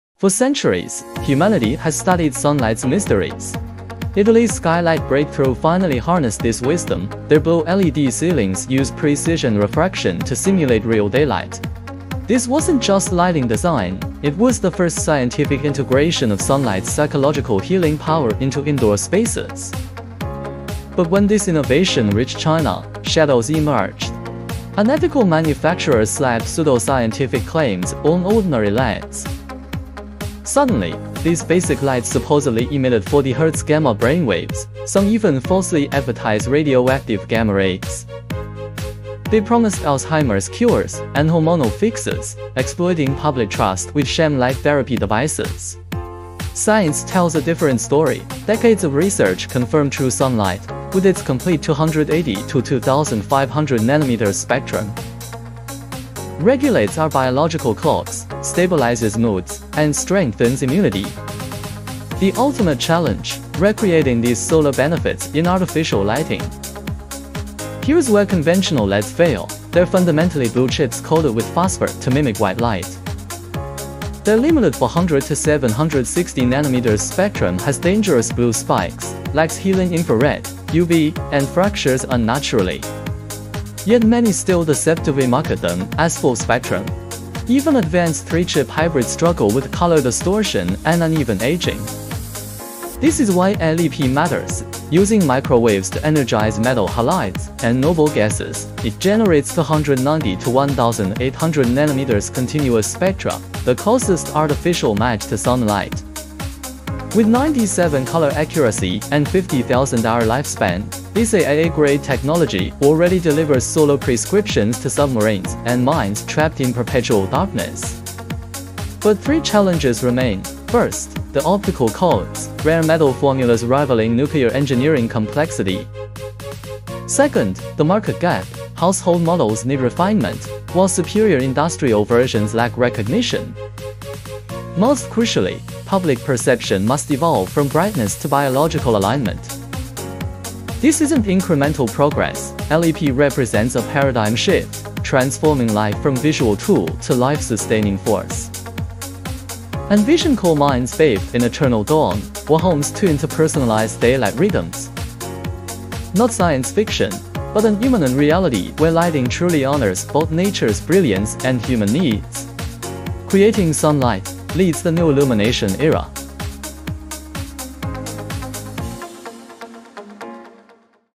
LEP Light source break the mold LEP Plasma light Light source
Plasma light source Lighting's Health Revolution

Let's start with Italy's groundbreaking skylight design. Their blue LED ceilings use light refraction to create realistic daylight illusions.
This wasn't just pretty lighting – it was the first time psychological healing through natural light entered interior design.

But when this concept reached China, something went wrong. Unethical manufacturers started slapping pseudoscience on ordinary LEDs.
Suddenly, these lights could emit 40Hz gamma brainwaves – some even lied about radioactive gamma rays!
They claimed cures for Alzheimer's and mental disorders, turning basic lights into expensive 'healing devices' – classic pseudoscientific scams.

Here's why this matters. Decades of research prove real sunlight – with its full spectrum from 280 to 2500 nanometers – regulates our sleep cycles, moods, and immunity. Recreating this became the lighting industry's ultimate challenge.

Let's expose LED limitations. They're fundamentally blue lights coated with phosphor to fake white light, missing crucial ultraviolet and infrared ranges.
Their 400-760nm spectrum has dangerous blue peaks and gaps, yet many still falsely market them as 'full-spectrum'.
Even hybrid UV/white/IR LEDs struggle with color inaccuracies and uneven brightness decay.

Enter LEP – microwave-powered plasma lighting. By exciting metal halides and inert gases, it creates 290-1800nm sunlight replica with 97 color accuracy rating. No bulbs to replace for 50,000 hours. This AAA-grade 'synthetic sun' now lights submarines and mines.

But three obstacles remain. First, the 'light recipe' using rare metals rivals nuclear engineering complexity. Second, home-use models are still underdeveloped while superior high-power versions lack public awareness. Finally, we need to shift from 'brightness obsession' to 'biological lighting' education.

This isn't just better bulbs. LEP represents a revolution – transforming lighting from visual tool to life-sustaining technology. Imagine eternal sunlight in deepest mines, or personalized daylight rhythms in every home. That's not sci-fi – it's the new era of light that truly nourishes life.